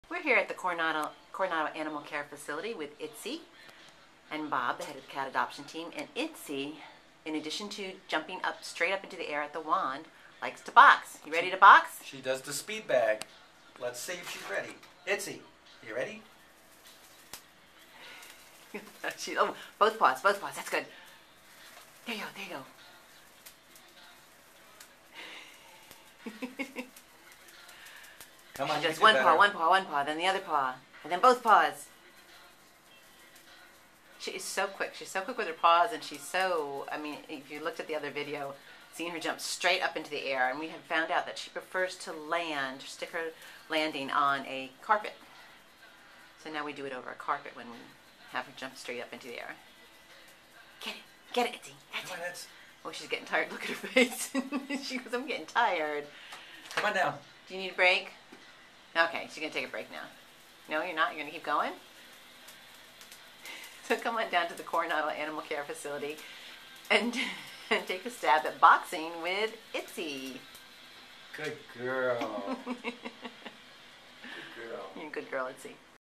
Itsy Boxing
You've seen the gymnastic talents of Itsy the beautiful Tuxedo cat for adoption from PAWS of Coronado where she jumps straight up into the air grabbing at a wand held six feet off the ground. Not only does this talented cat tap the wand, she always sticks a purrfect landing!

Now watch her boxing skills as she boxes with the wand. Left jabs flying, then right jabs, then a repeated left-right combo. All done with amazing speed.

Itsy is one very talented cat - who knows what skills we will uncover next!